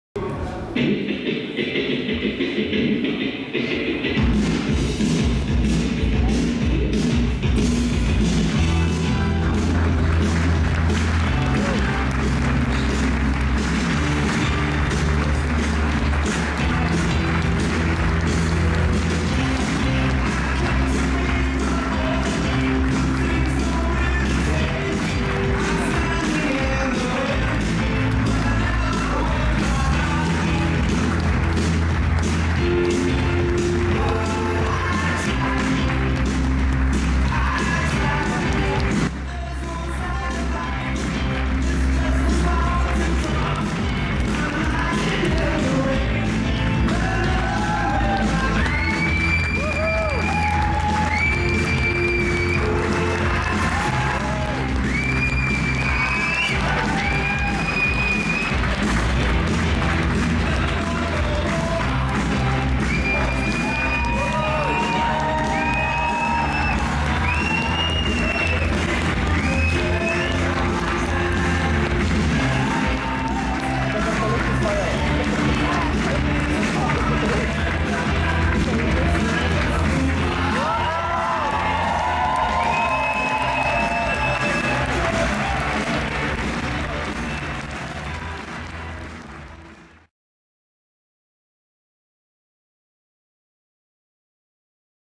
RAF SIMONS FINALE AW 2013-2014
Raf Simons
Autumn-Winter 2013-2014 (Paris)
FINALE

View from the audience (filmed with a handy cam)